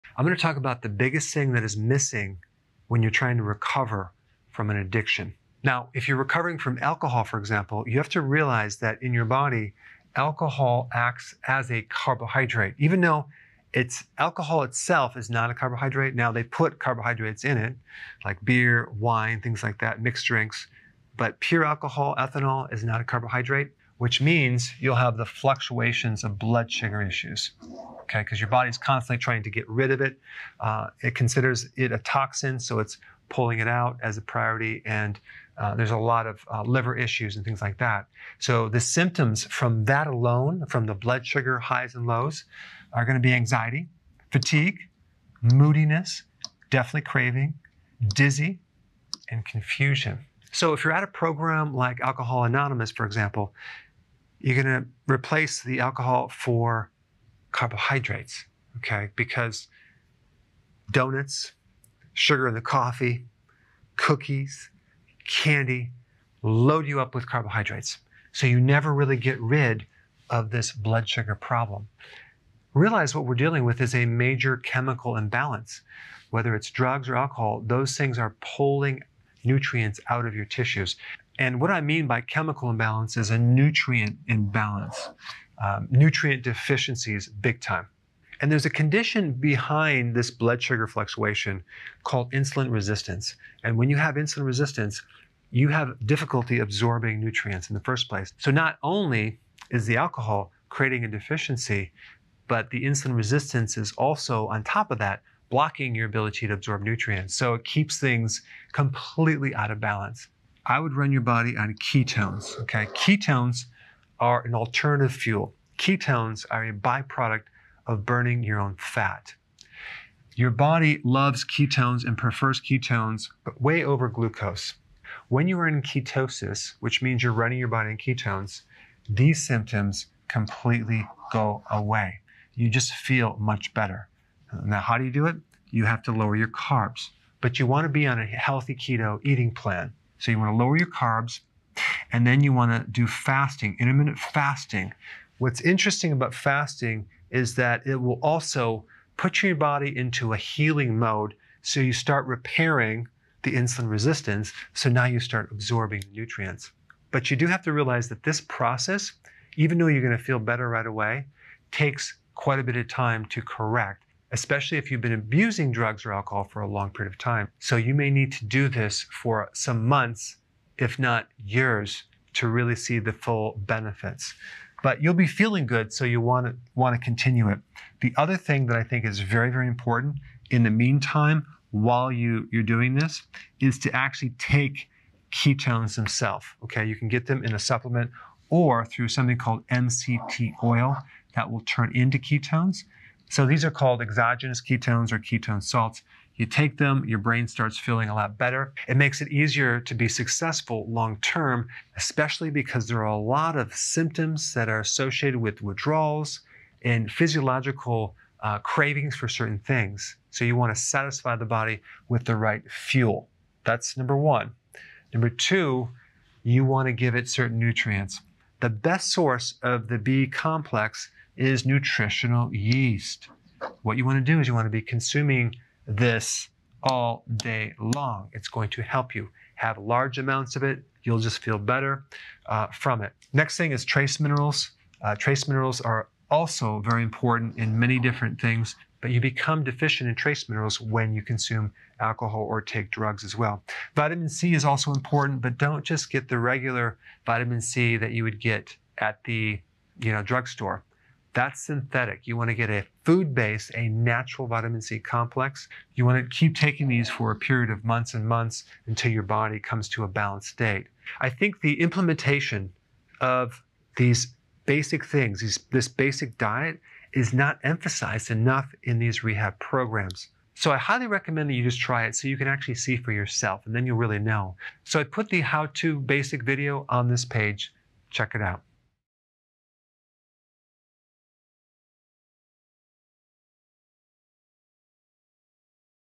The #1 Thing Missing in Addiction Recovery
What is missing from addiction recovery? Watch this video to find out.

Timestamps:
0:00 What is missing from addiction recovery
0:27 Problems with blood sugar
0:43 Symptoms of blood sugar issues
1:50 Why you should run on ketones
2:18 How to heal insulin resistance
3:35 How to replenish nutrients

In this video, I want to talk to you about the biggest thing that is missing from addiction recovery.

If you’re recovering from alcohol, for example, you have to realize that alcohol acts like a carbohydrate in the body. However, alcohol itself isn’t a carbohydrate, which means that you’ll have the fluctuations of blood sugar issues.

The symptoms of these blood sugar highs and lows are:
• Anxiety
• Exhaustion
• Moodiness
• Cravings
• Dizziness
• Confusion

When you are at something like alcoholics anonymous, for example, you are going to replace alcohol with carbs and sugars—think doughnuts, cookies, and sweetened coffee.

What we’re really dealing with are nutrient deficiencies. Addictive substances pull nutrients out of your tissues.

As your blood sugar fluctuates, you develop something called insulin resistance. When you have insulin resistance, you have a difficult time absorbing nutrients in the first place.

So not only is the alcohol (or other substance) causing nutrient deficiencies directly, it also causes nutrient deficiencies by contributing to insulin resistance.

The best things to help combat these problems are:
• Keto
• Intermittent fasting
• MCT oil
• Nutritional yeast
• Trace minerals
• Vitamin C

I hope this video helps you have a better understanding of what you can do to help you or a loved one get through addiction recovery.